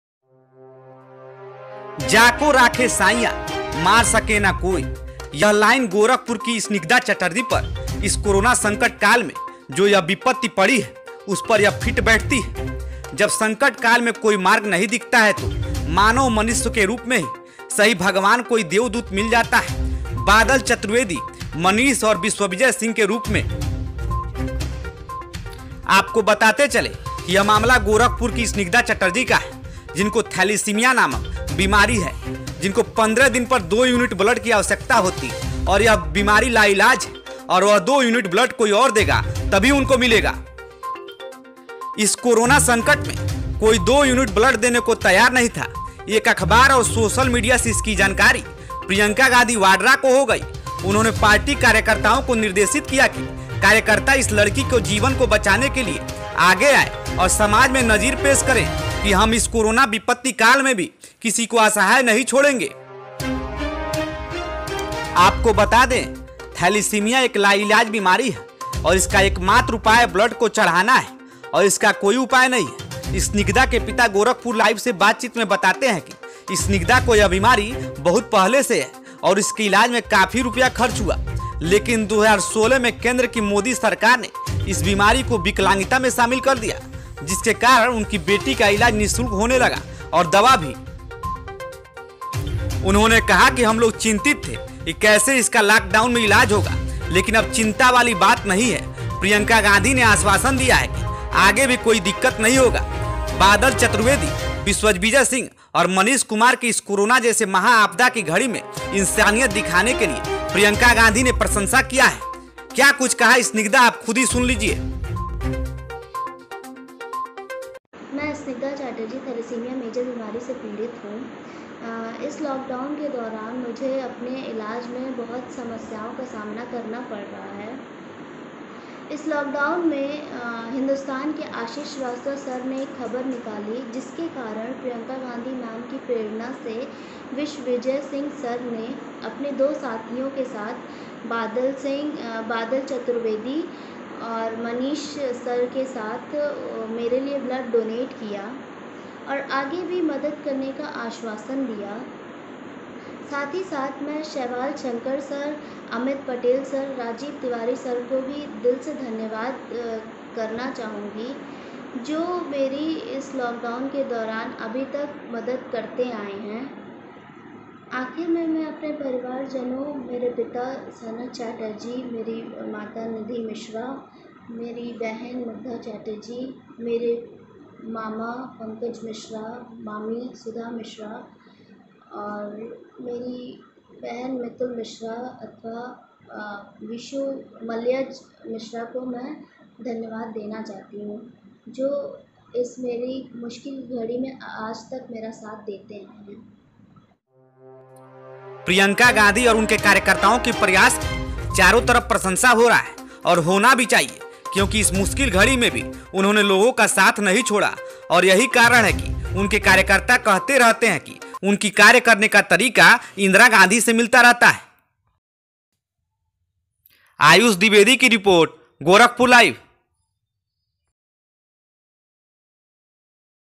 प्रियंका गांधी केअथक प्रयास से lockdown में गोरखपुर के एक लड़की को जीवनदान मिली
Priyanka gandhi ,gorakhpur